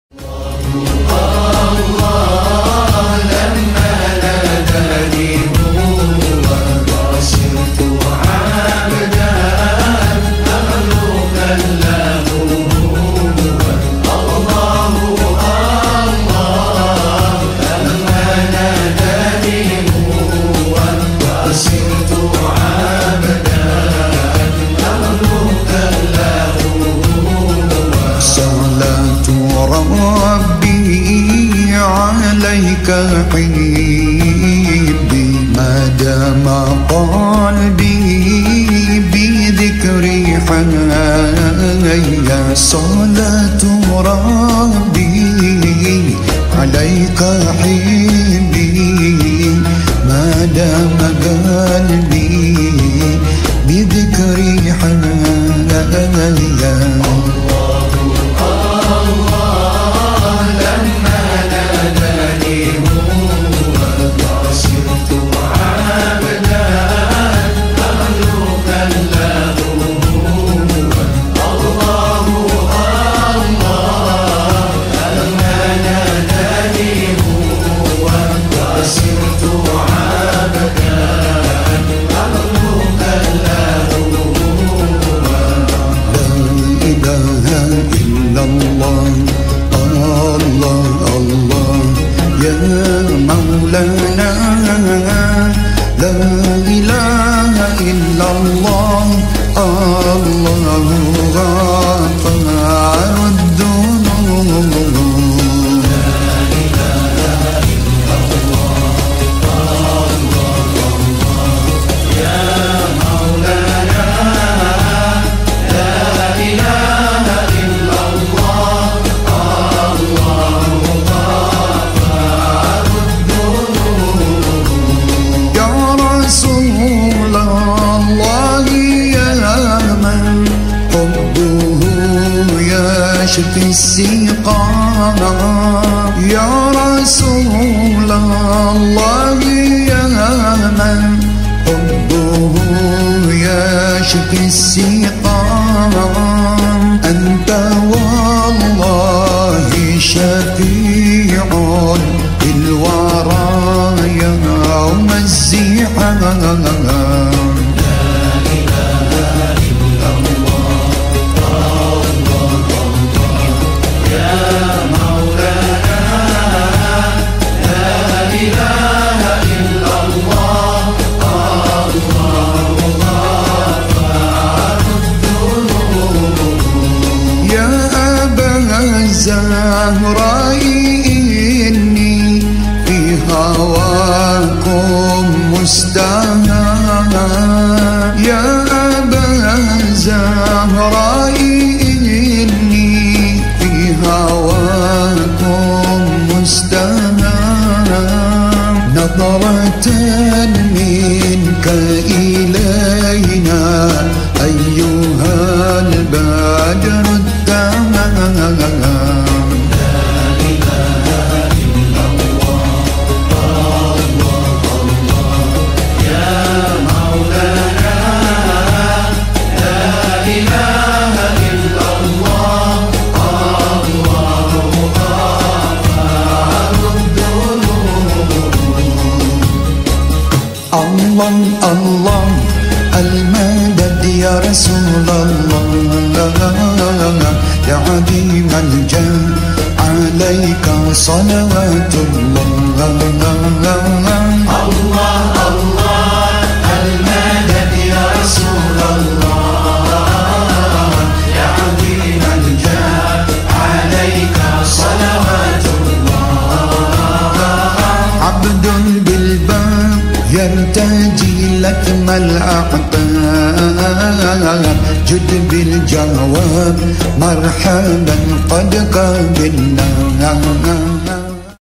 Ziarah makam Waliyullah, Demak & Kudus + MAJT 2019
Rutinitas Tahunan PP. Al Manshuriyah Mengori Pemalang